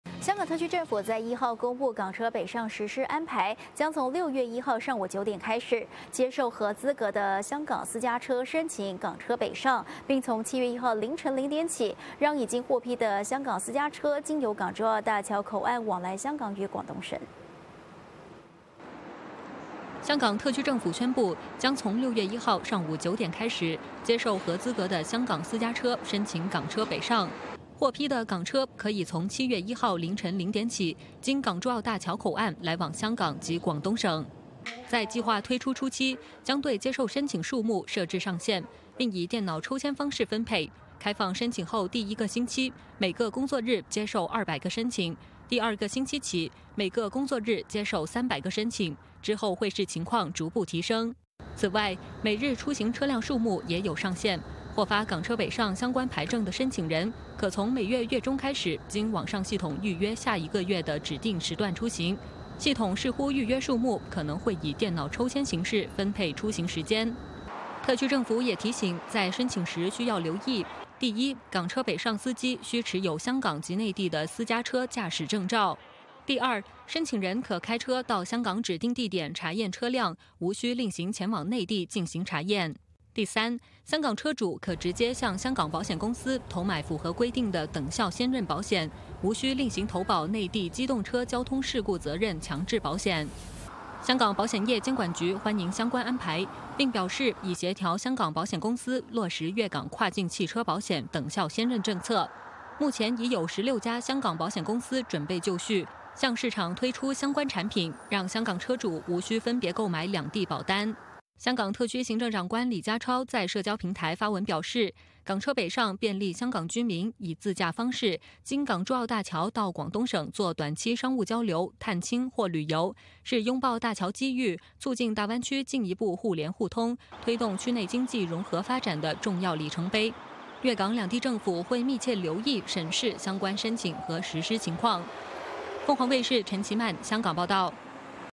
【“港車北上”】6月起接受申請 7月起實施通行
香港特區政府1日公佈“港車北上”實施安排，將從6月1日上午9點開始，接受合資格的香港私家車申請“港車北上”，並從7月1日凌晨零點起，讓已獲批的香港私家車，經港珠澳大橋口岸往來香港與廣東省。